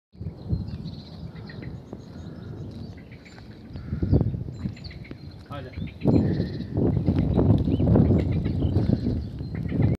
The cleanest Lake in Albania 🇦🇱
The water is very clean you can se everything in lake,is the best place to swimming and relax yourself is in the middle of trees 🍀🍀🌴